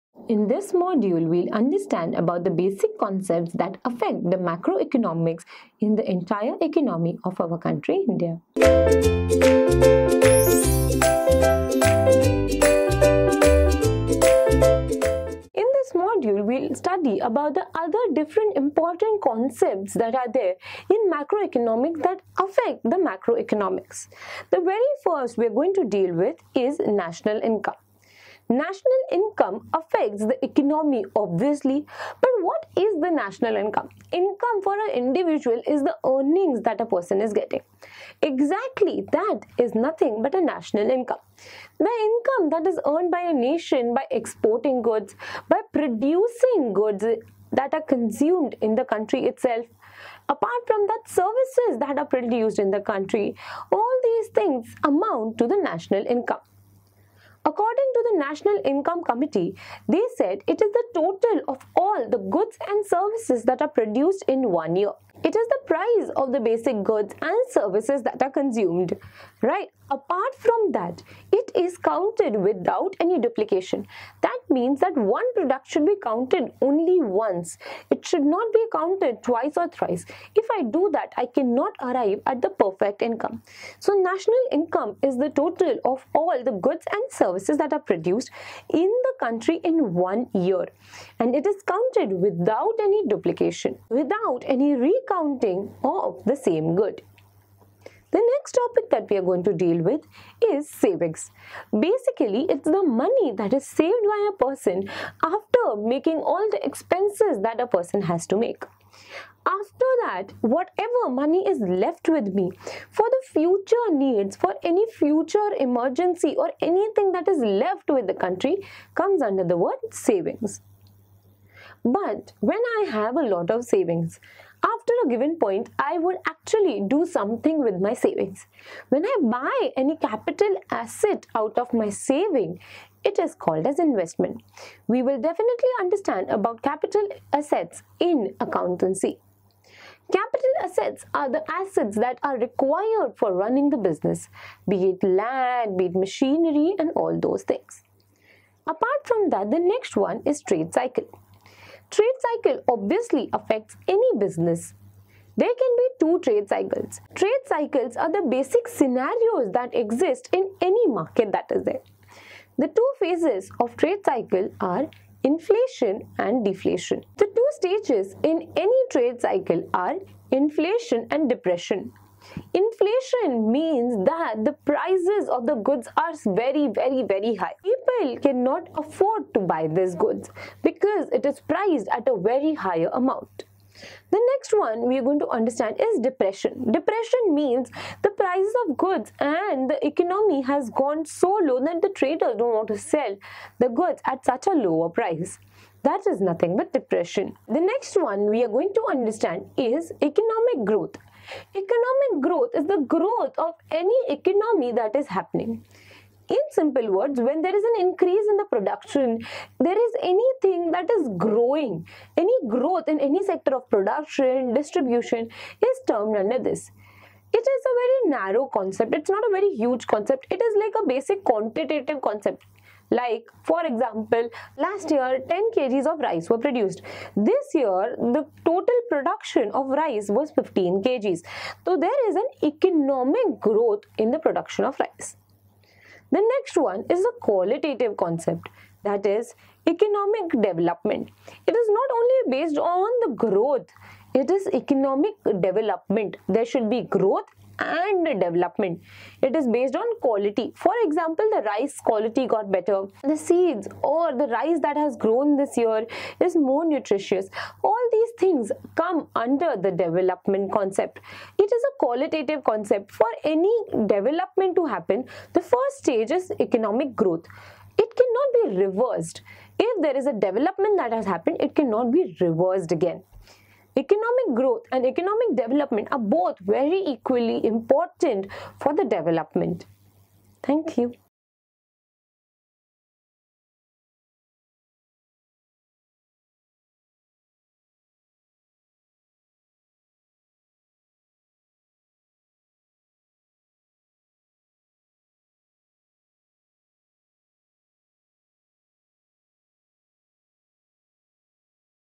Basic Concepts of Macroeconomics - Basic Concept in Economics - Economics
Subject - Economics

Video Name - Basic Concepts of Macroeconomics

Chapter - Basic Concept in Economics

Faculty - Prof. Rutu Gori

Watch the video lecture on topic Basic Concepts of Macroeconomics of Subject Economics by Professor Rutu Gori. Watch all Commerce and Management lectures with Ekeeda.

Happy Learning.